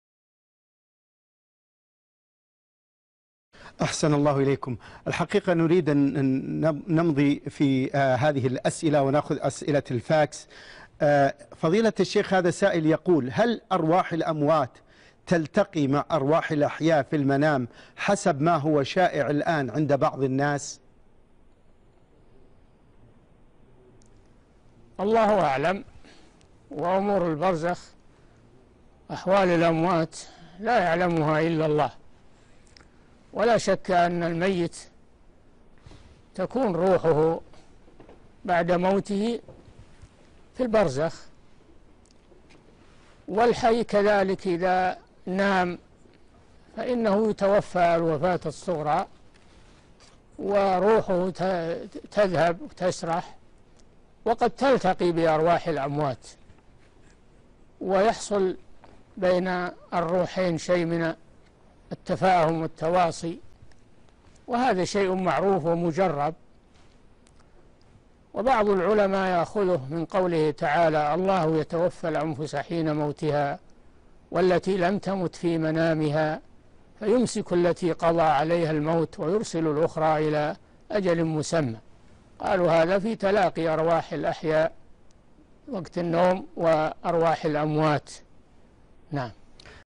حقيقة التقاء أرواح الأحياء والأموات في المنام - الشيخ صالح الفوزان
نص السؤال: هذا سائل يقول: هل أرواح الأموات تلتقي مع أرواح الأحياء في المنام حسب ما هو شاع الآن عند بعض الناس؟ رابط الحلقة كاملة
السيرة الذاتية :
للاشتراك في قناة الشيخ صالح الفوزان
لمشاهدة حلقات فتاوى على الهواء كاملة

رئيس الفريق
لمشروع كبار العلماء